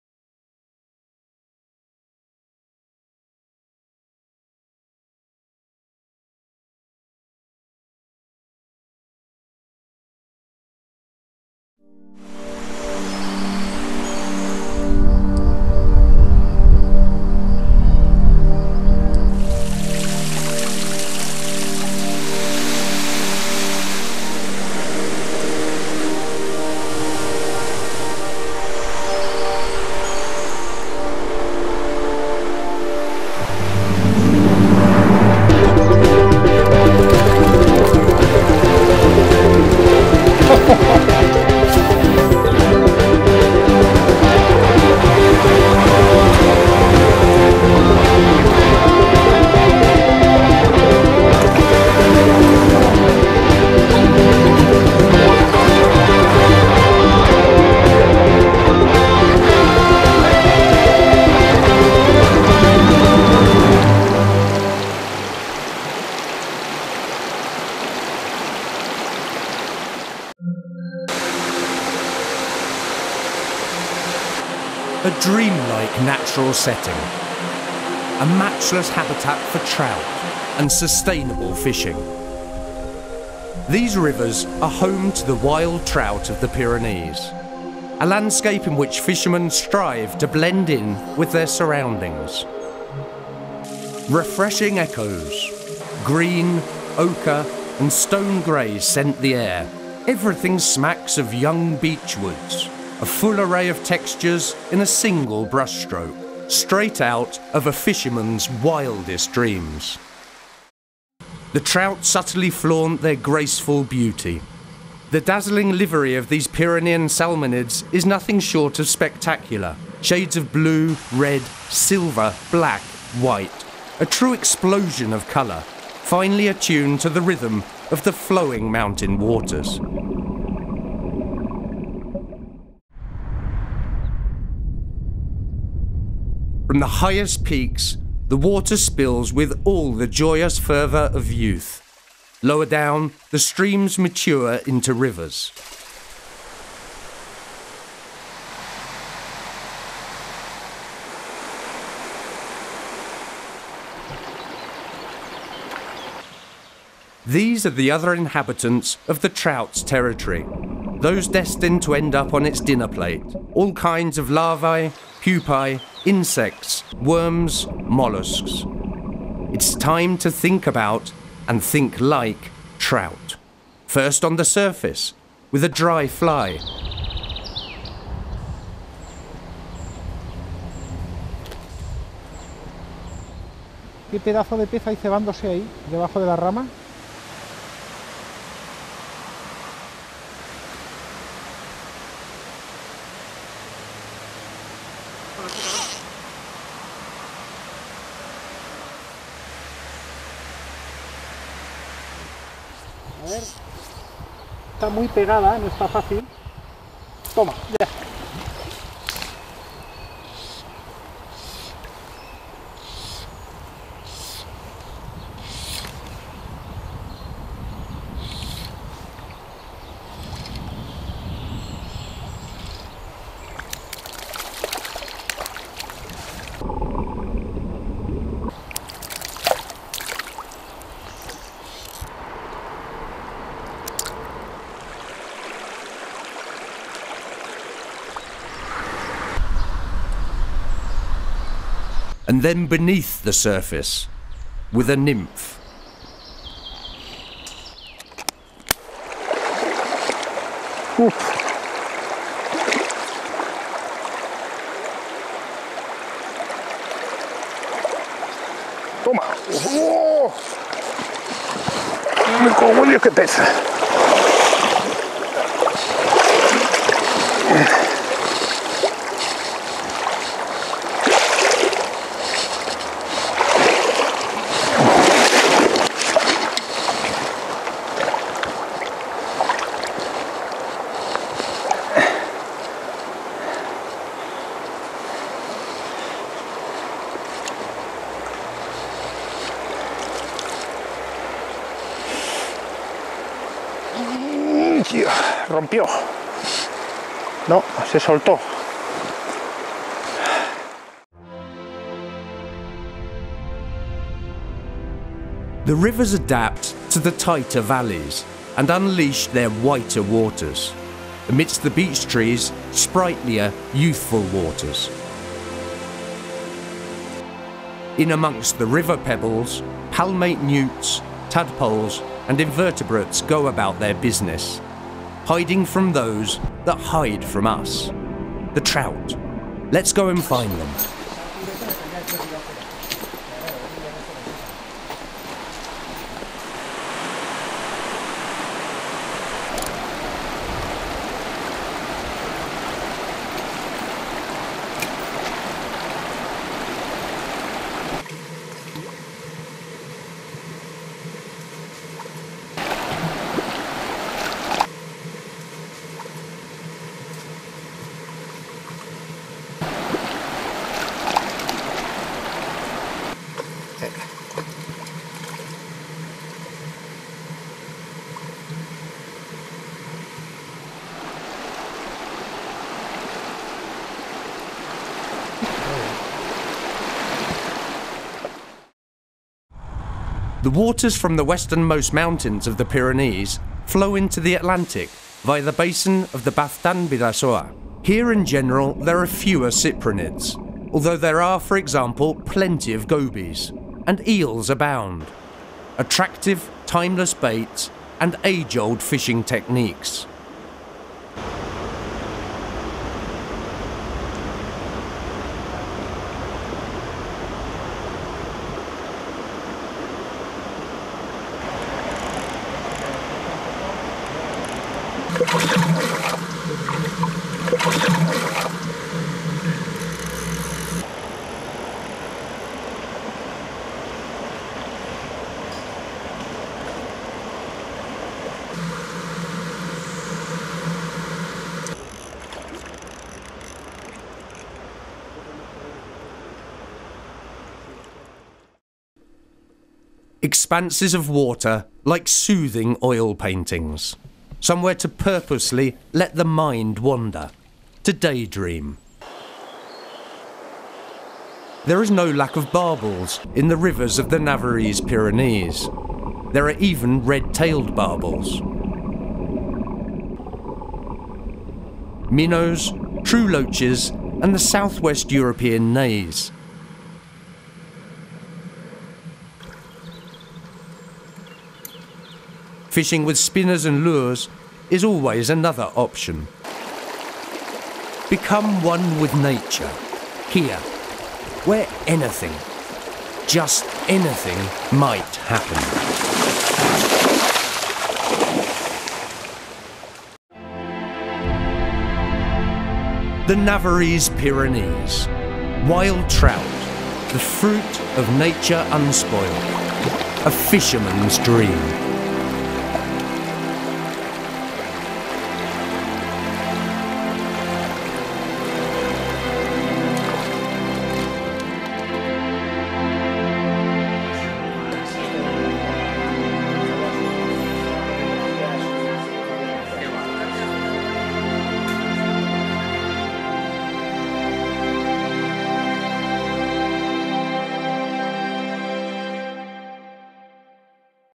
Trout Fishing Navarre
Fishing for wild trout in Navarra, at the Pyrenees mountains. The fisherman merges with the natural environment and emulates the most bucolic passages from the book "The sun also rises", by Ernest Hemingway:

(...) Bill put the pack against one of the trees and we jointed up the rods, put on the reels, tied on leaders, and got ready to fish.
"You're sure this thing has trout in it?" Bill asked.
"It's full of them." (..)